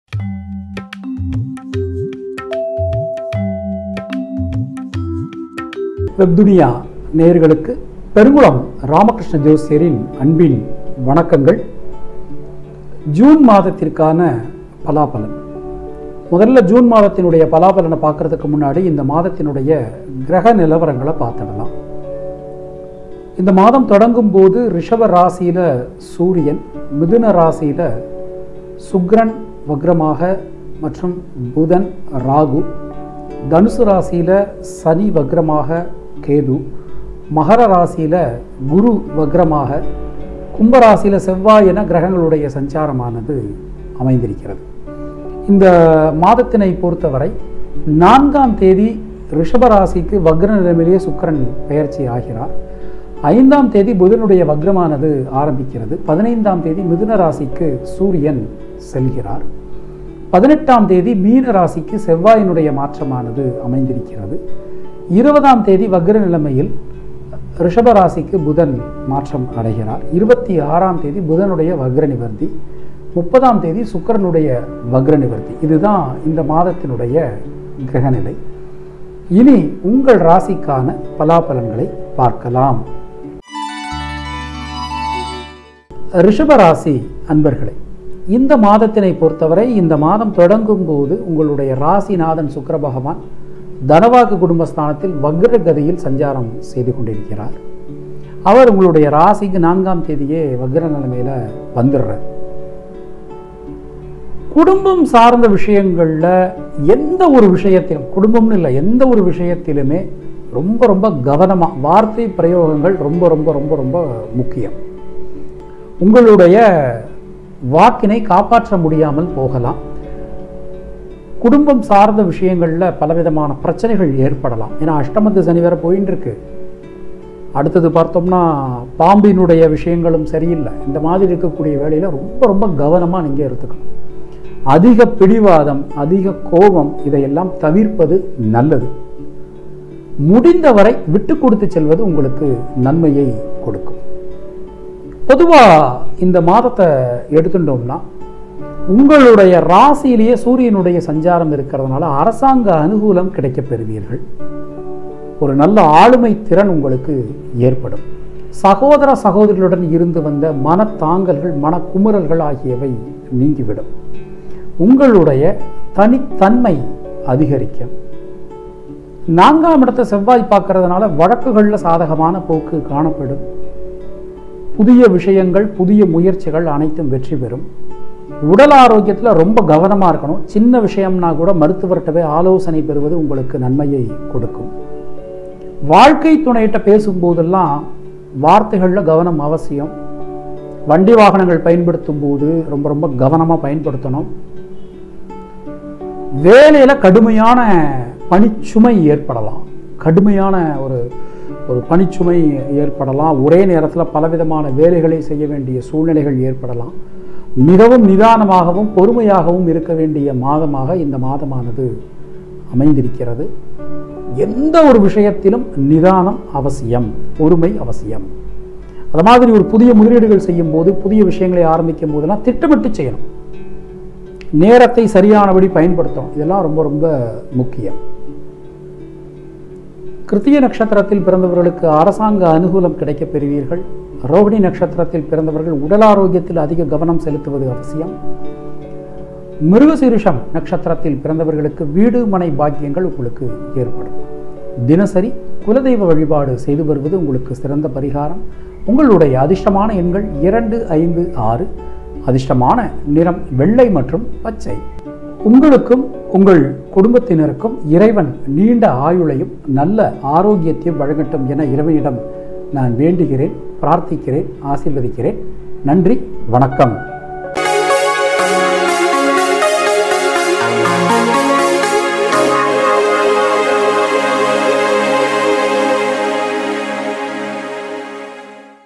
Rishabam Rasi | June Rasi palan 2020 | Taurus | ரிஷபம் ராசி | ஜூன் மாத பலன்கள் 2020
ஜூன் மாத பலன்கள் 2020 கணித்து, வழங்குபவர் பெருங்குளம் ராமகிருஷ்ண ஜோஸ்யர்.

ஆன்மிகத்தில் உங்கள் சந்தேகங்கள் குறித்த தொடர்புக்கு